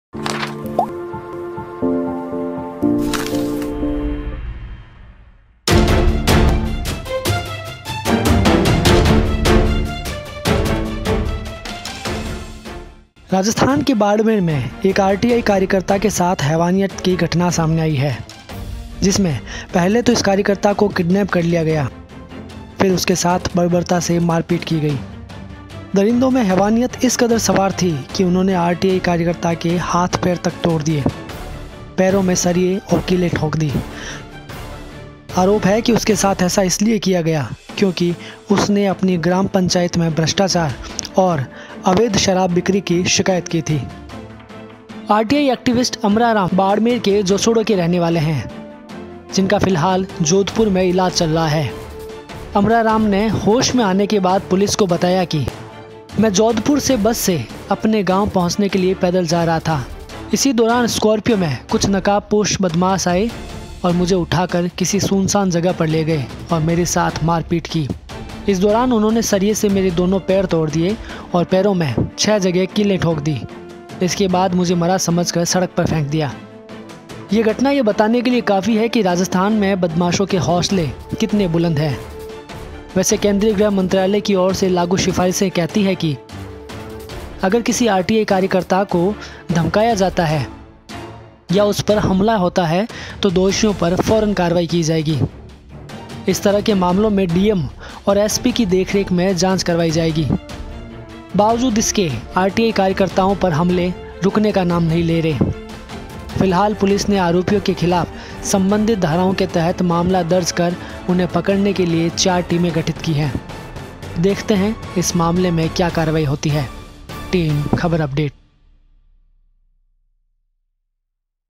बदमाशों ने पहले तो RTI कार्यकर्ता के हाथ-पैर तोड़े फिर पैरों में कीलें ठोंक दी| Khabar Update
KhabarUpdate, राजस्थान के बाड़मेर में एक आरटीआई कार्यकर्ता के साथ हैवानियत की घटना सामने आई है. जिसमें पहले तो इस कार्यकर्ता को किडनैप कर लिया गया, फिर उसके साथ बर्बरता से मारपीट की गई। दरिदों में हैवानियत इस कदर सवार थी कि उन्होंने आरटीआई कार्यकर्ता के हाथ-पैर तक तोड़ दिए, पैरों में सरिये और कीलें ठोंक दी. आरोप है कि उसके साथ ऐसा इसलिए किया गया क्योंकि उसने अपनी ग्राम पंचायत में भ्रष्टाचार और अवैध शराब बिक्री की शिकायत की थी.